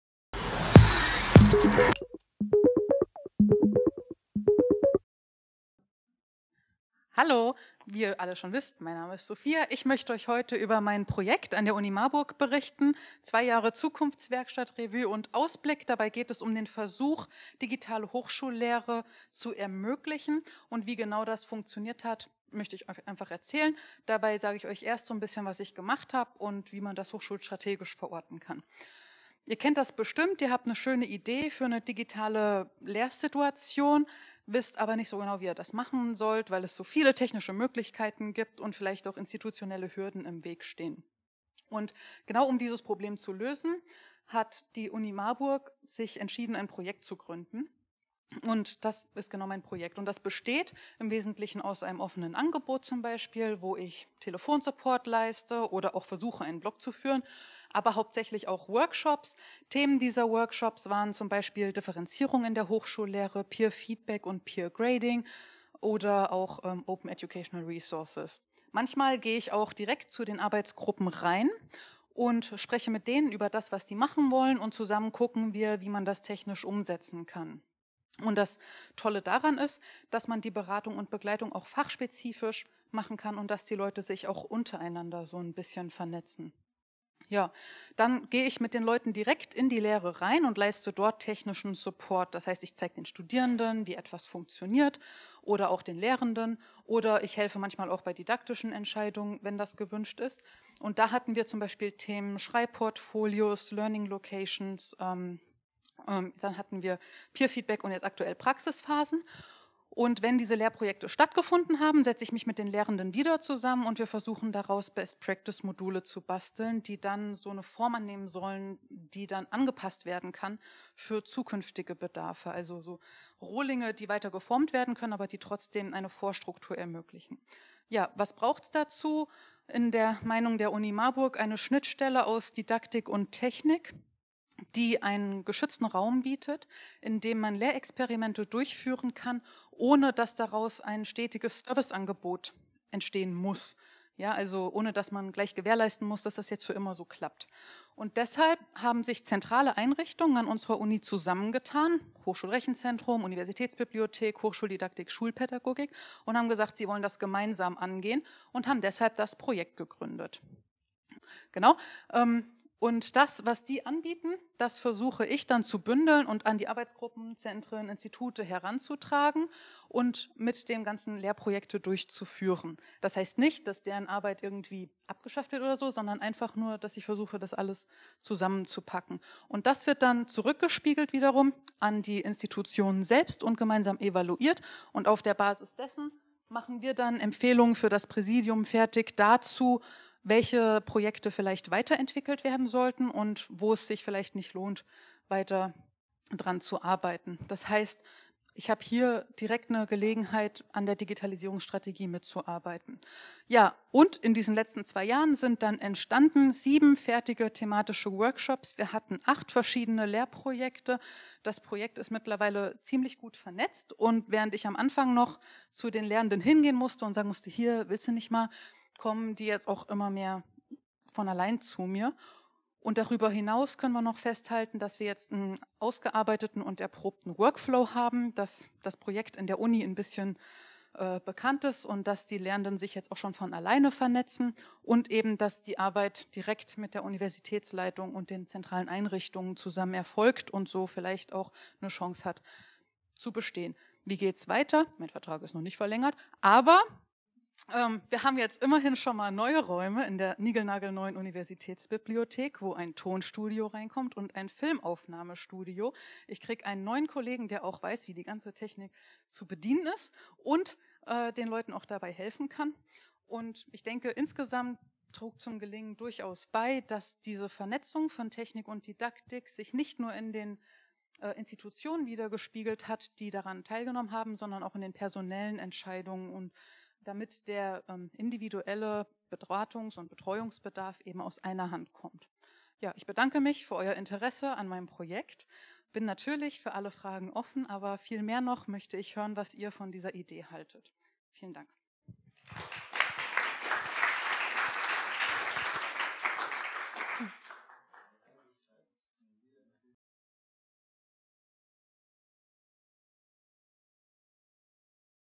Zwei Jahre Zukunftswerkstatt für digitale Hochschullehre - Sophia Hercher
Das Projekt Zukunftswerkstatt an der Uni Marburg hat als Schnittstelle zwischen technischer und didaktischer Beratung seit knapp zwei Jahren die Aufgabe, Lehrende dazu zu ermutigen, digitale Technologien einzusetzen und auch ungewöhnliche Formate und Technologien in einer geschützten Umgebung zu ermöglichen. Wie das bisher gelungen ist und was die Zukunft bringen muss, damit ein solches Format an einer Universität funktionieren kann, hat Sophia Hercher in ihrem Lightning Talk vorgestellt werden.